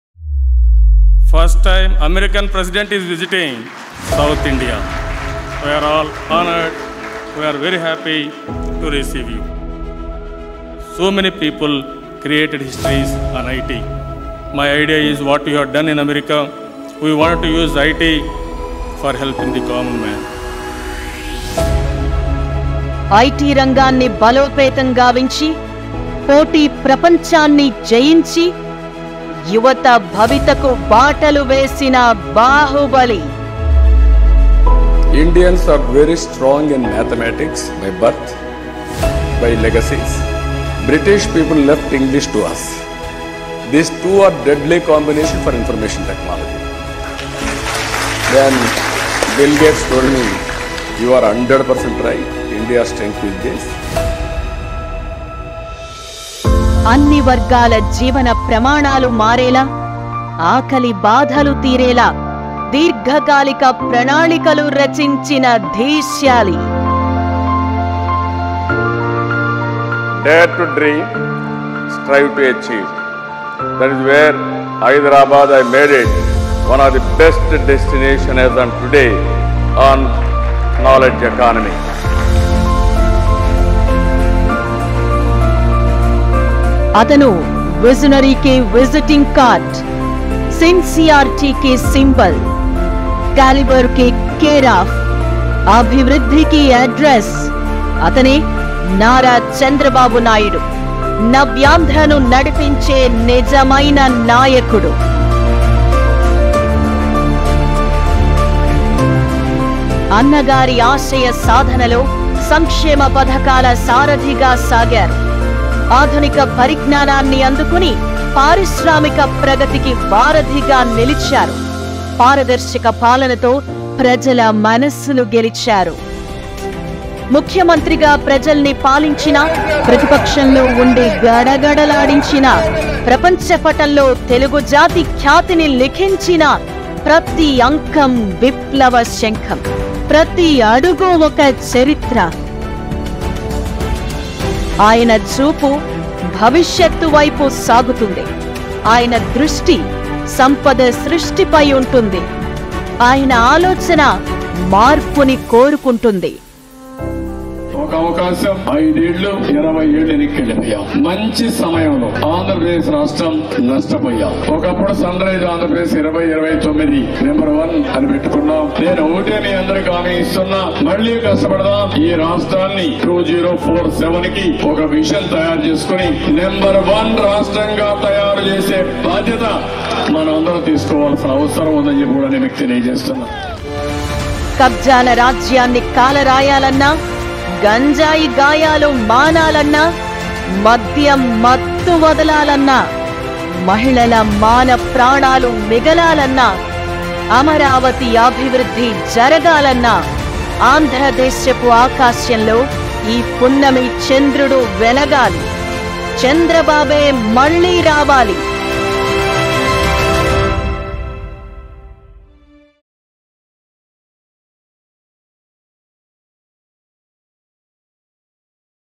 Chandrababu Naidu The Visionary Leader of Andhra Pradesh | Special Story | TDP
Chandrababu Naidu The Visionary Leader of Andhra Pradesh. Chandrababu Naidu was an exemplary leader dedicated to the development of the unified State of Andhra Pradesh and the welfare of the masses and continued to strive for the well-being of the residuary State of A.P. AP Elections To Be Held On May 13th, 2024.